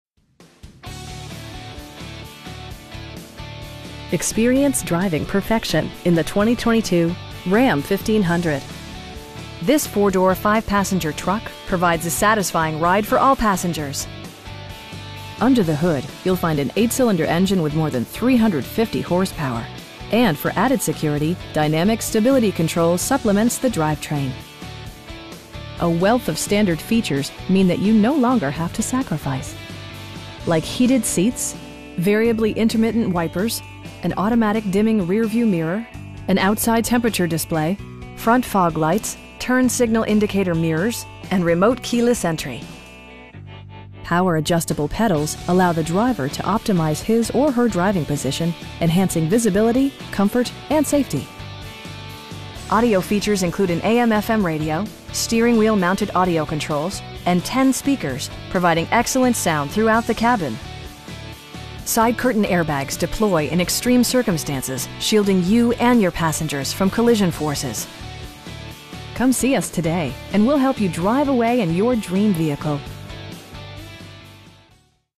2022 Ram 1500 LONE STAR QUAD CAB 4X2 6'4 BOX
All American Chrysler Dodge Jeep Ram FIAT of San Angelo

Experience driving perfection in the 2022 Ram 1500 This 4 door, 5 passenger truck provides a satisfying ride for all passengers. Under the hood you'll find an 8 cylinder engine with more than 350 horsepower, and for added security, dynamic Stability Control supplements the drivetrain. A wealth of standard features means that you no longer have to sacrifice. Like heated seats, variably intermittent wipers, an automatic dimming rear-view mirror, an outside temperature display, front fog lights, turn signal indicator mirrors, an overhead console, and remote keyless entry. Power adjustable pedals allow the driver to optimize his or her driving position, enhancing visibility, comfort and safety. Audio features include an AM/FM radio, steering wheel mounted audio controls, and 10 speakers, providing excellent sound throughout the cabin. Side curtain airbags deploy in extreme circumstances, shielding you and your passengers from collision forces. Come see us today and we'll help you drive away in your dream vehicle!